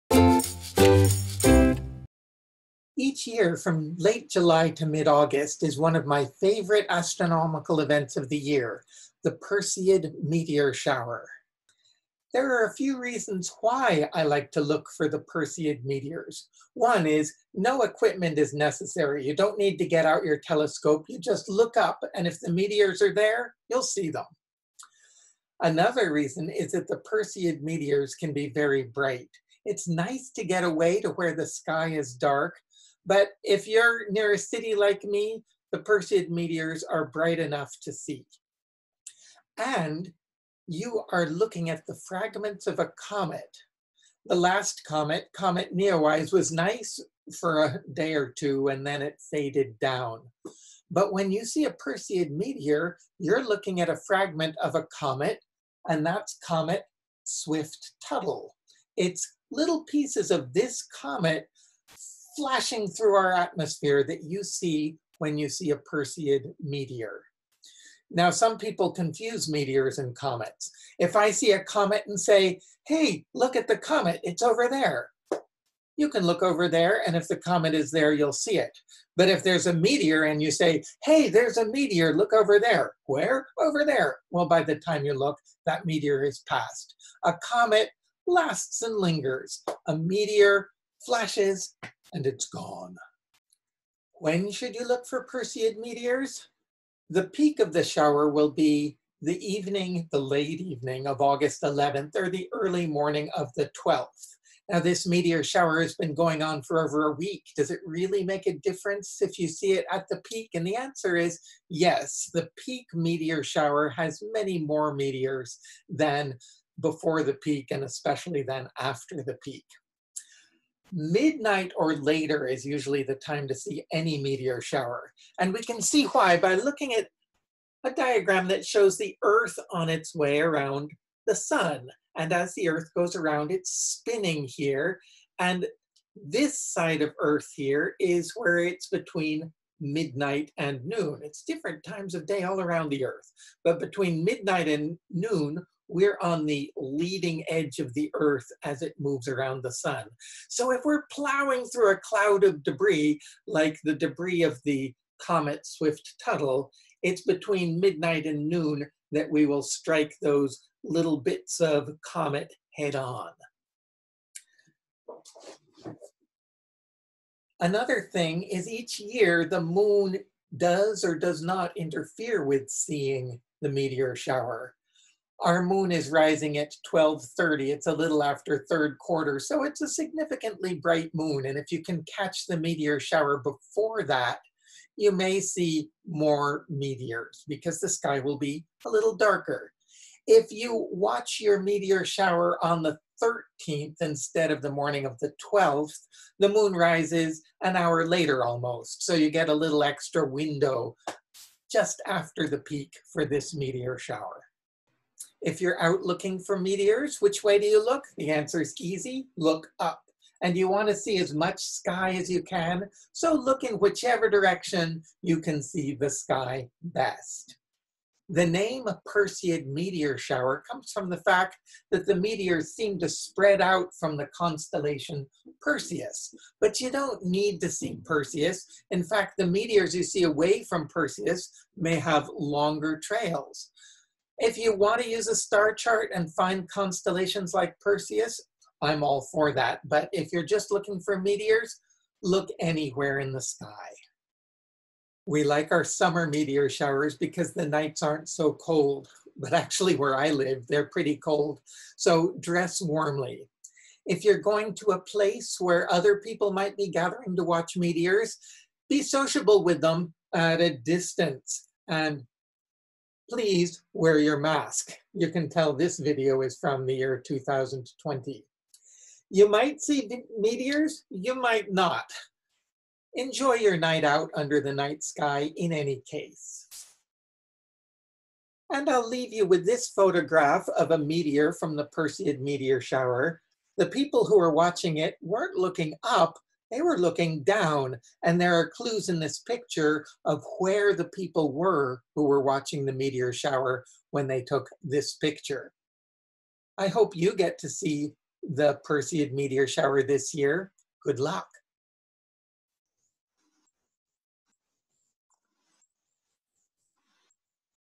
Perseid Meteor Shower Best on August 11-12: A Search the Sky Activity
A streak of light crosses the sky, and fades in about a second. Wait a bit...Will there be another? Ooh, there it is. It’s the peak of Perseid Meteor shower, the best shower of the year in the Northern Hemisphere. Learn Why, When, Where, and How to see it. What will it look like to you? If the weather is clear, find out!